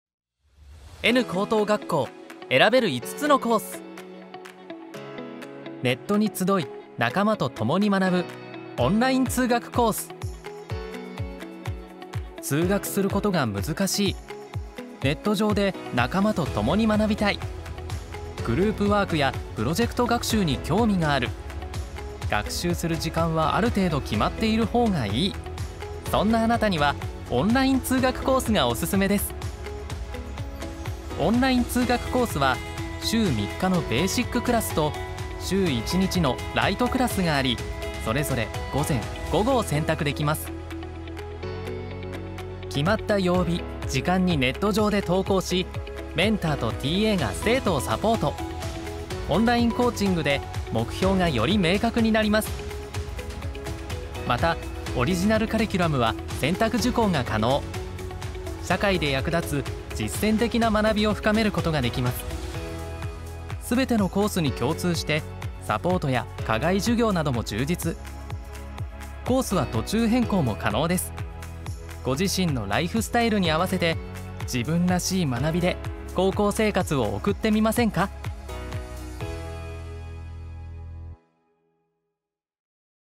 【コース紹介】オンライン通学コース丨ネットに集い仲間と共に学ぶ N高等学校・S高等学校
N高等学校・S高等学校では、生徒の目標やライフスタイルに合わせて、自分らしい学習ができるように5つのコースを用意しています。

自分のペースでネットで学んでもいいし、キャンパスに通学して仲間と学んでもいい。
コースは途中変更も可能です。

どのコースを選択しても必修授業（ネット学習・スクーリング・テスト）を学習することで、高校卒業資格を取得することができます。

■N高等学校・S高等学校 公式リンク

N高・S高のパンフレット（学校案内・コース紹介）と募集要項などを無料でお届けします。

★入学相談窓口はコチラ★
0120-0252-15　（平日10時〜19時）